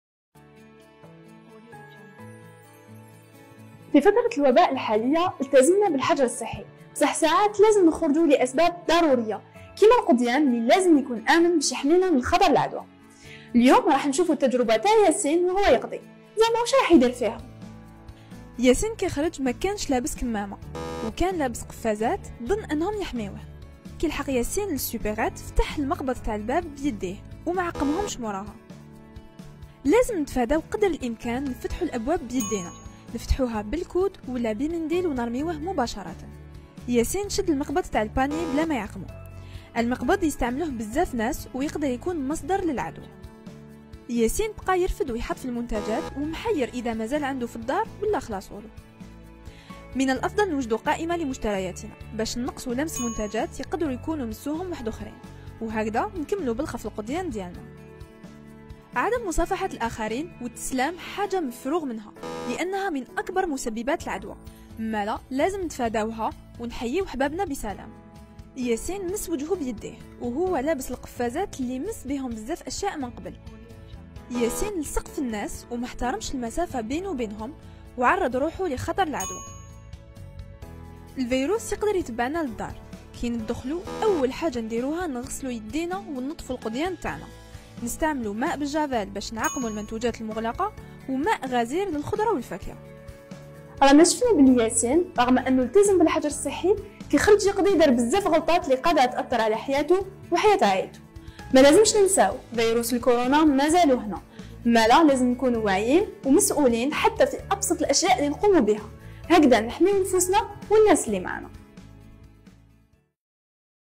التسوق في زمن الكورونا : التدابير التي يجب أخذها و الأخطاء التي يجب تفاديها
مالازمش ننساو ❗ فيروس كورونا مزالو هنا 🦠
لازم نكونو واعيين و مسؤولين حتى في أبسط الأشياء لي نقومو بها هكذا نحمو نفوسنا و الناس لي معانا 👨‍👩‍👧‍👦